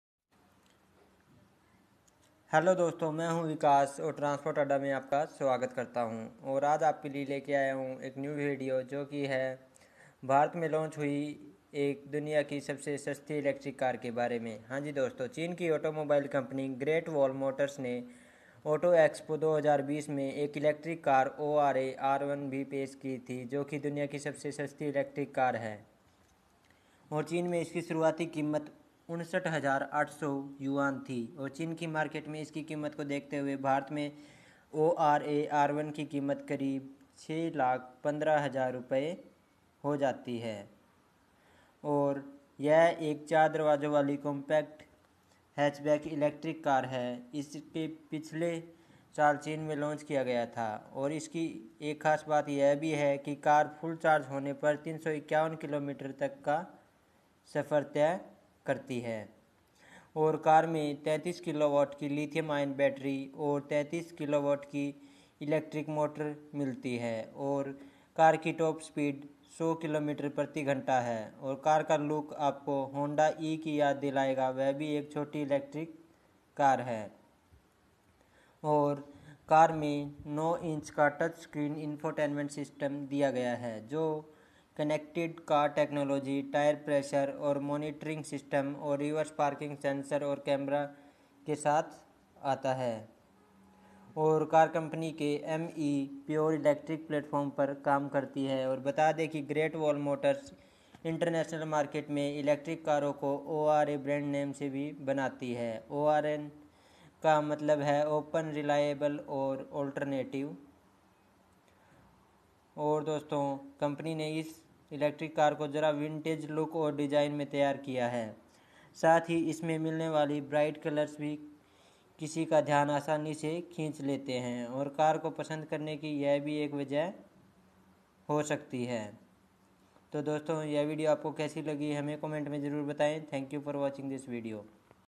सबसे सस्ती इलेक्ट्रिक कार l GWM R1 Price in India l Cheapest Electric Car Launched in India l
What is the expected price of ORA R1?
What is the launch date of ORA R1?
What are the expected key specifications of ORA R1?

Electric motor: 35 kW (48 PS; 47 bhp) permanent magnet motor
Electric range: 301 km (187 mi) (NEDC)
Battery: 33 kWh lithium polymer (LMP) battery

Chinese carmaker Great Wall Motors (GWM) will soon launch what is being touted as the world’s cheapest electric car in India.

New technology never comes cheap, and none more so in the world of electric vehicles, thanks to the price of batteries.

But with the confirmation from Chinese auto giant Great Wall Motors (which also owns the Haval, Ora and Wey brands) that it will enter the Indian market and be present at the country’s upcoming Auto Expo (as reported by the India Times), comes news that it may be planning to introduce the 4-door Ora R1 with 351km range and a 35kWh battery.